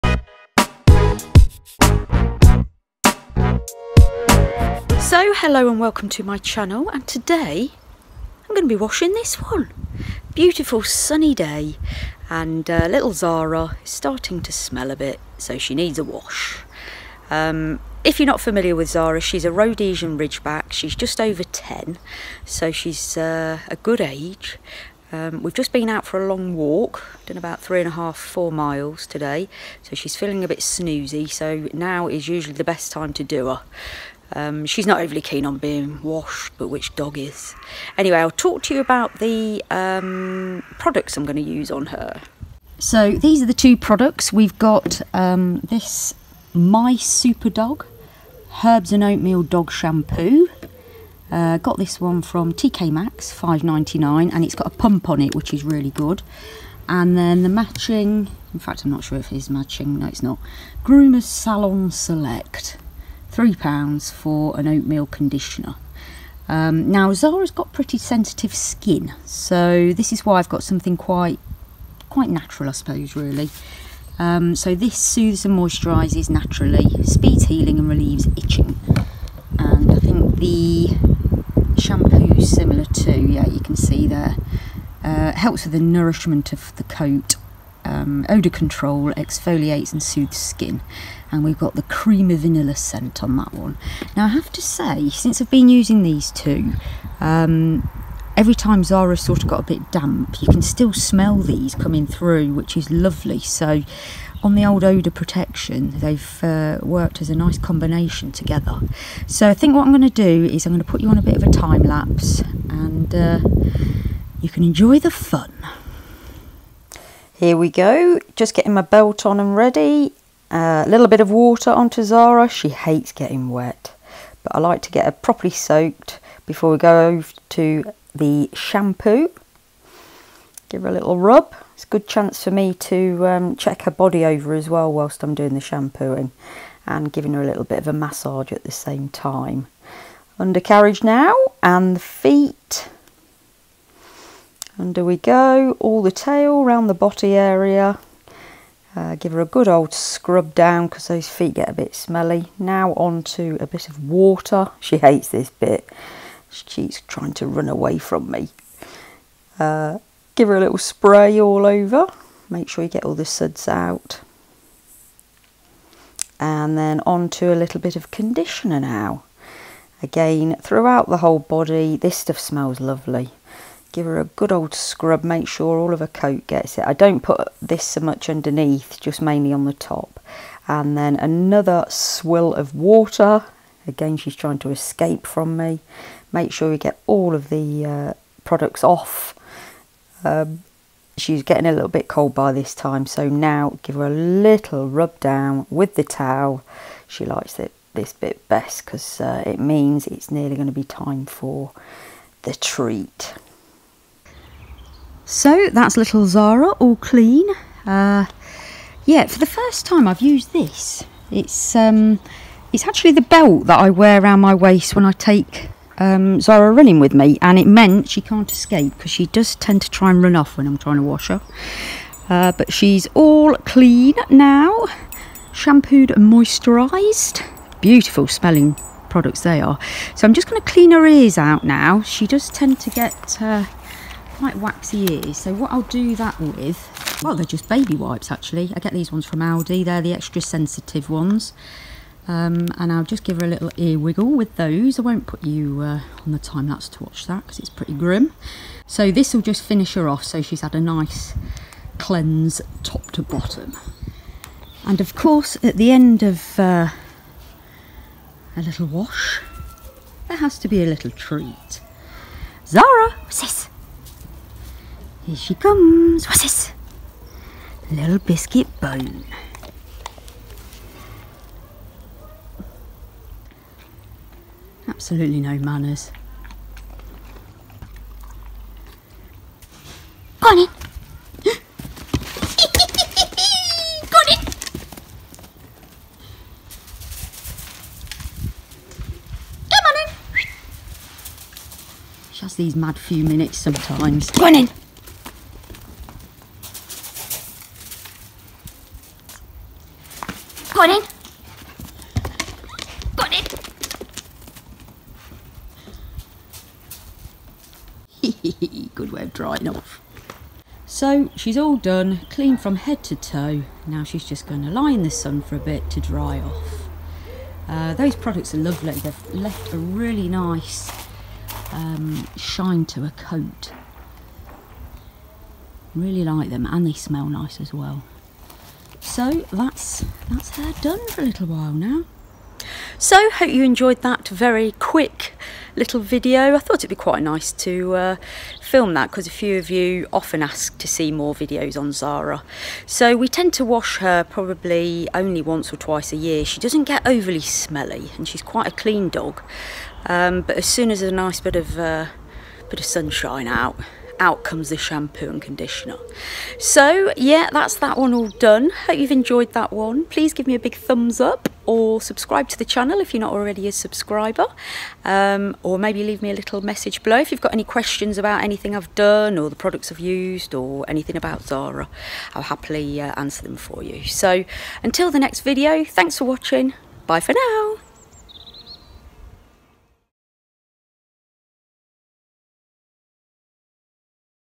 Zara the Rhodesian Ridgeback - Top to Tail Shampoo & Set - Dog Groom
It’s that time of year for our beautiful Rhodesian Ridgeback, Zara, to have a freshen up. The products I use which I bought from TK Maxx give her coat a beautiful shine and a lovely smell ☺️

I love doing product reviews -